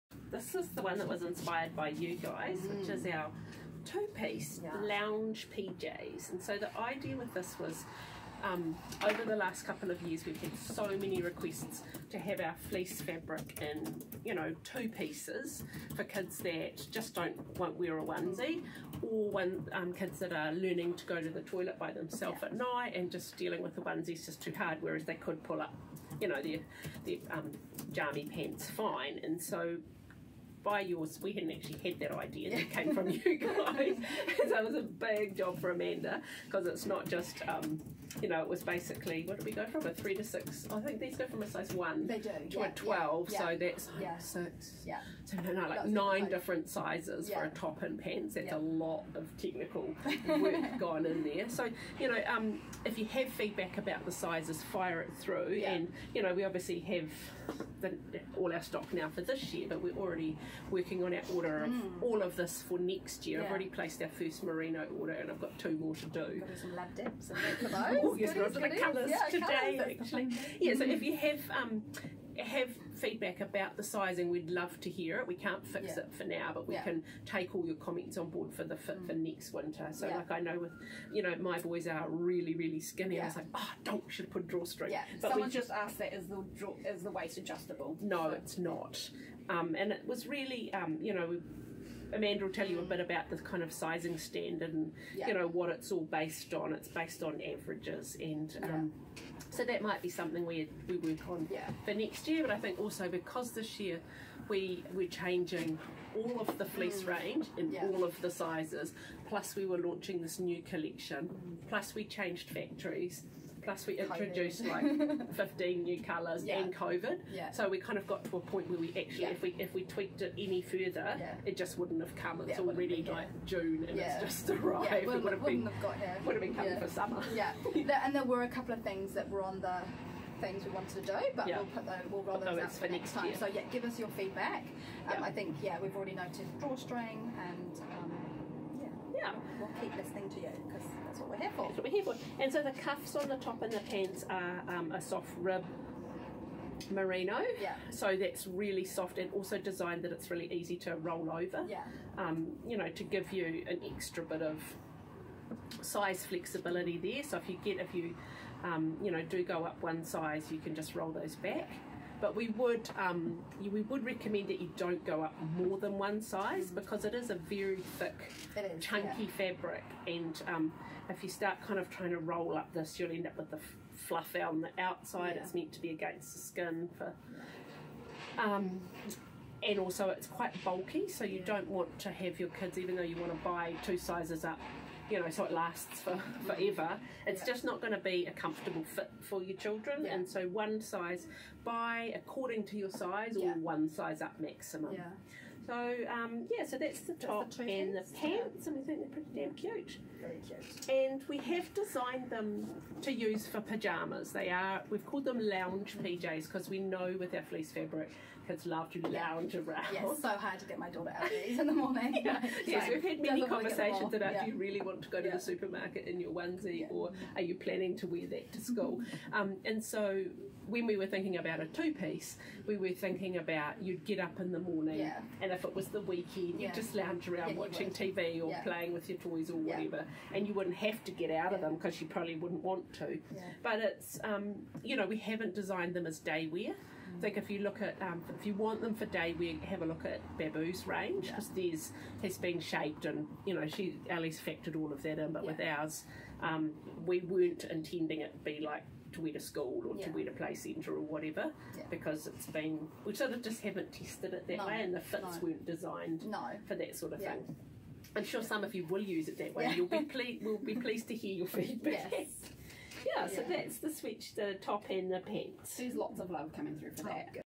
The Sleep Store 2 Piece Deluxe Merino Fleece Lounge Pyjamas
Lou and Amanda showcase our beuatiful new The Sleep Store Merino Fleece Lounge Pyjamas. What sizes are available, what ages are they most suited for, what colours are available?